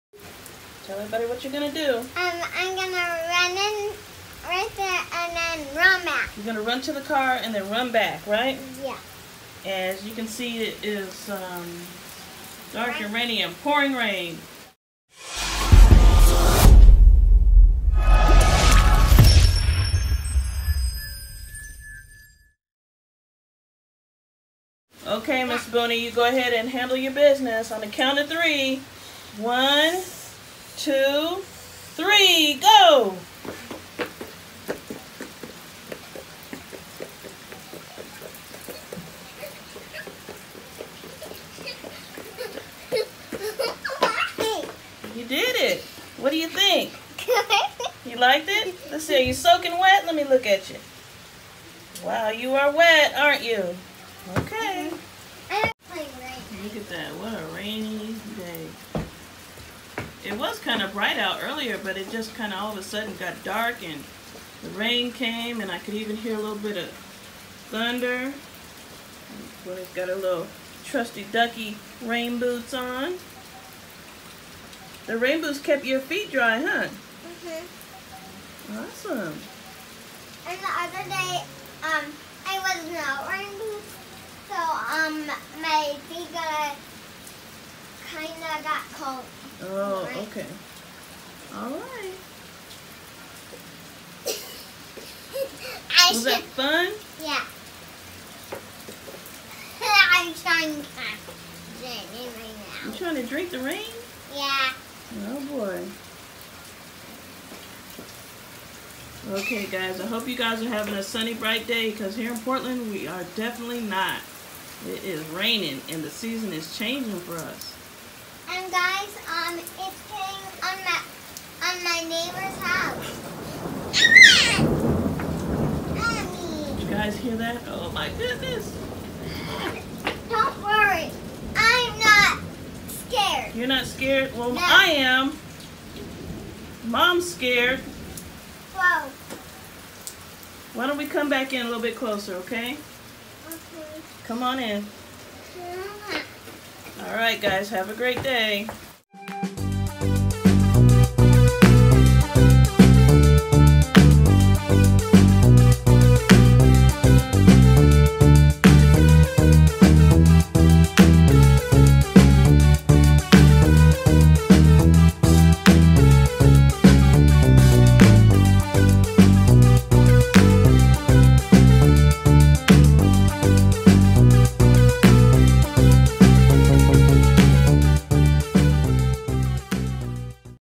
**KIERAN’S RAINY DAY CHALLENGE**
Today was such a rainy autumn day, and Mom and Kieran were starting to get a touch of cabin fever. So we decided to have a little fun and create a little rainy day challenge with a little surprise in the process.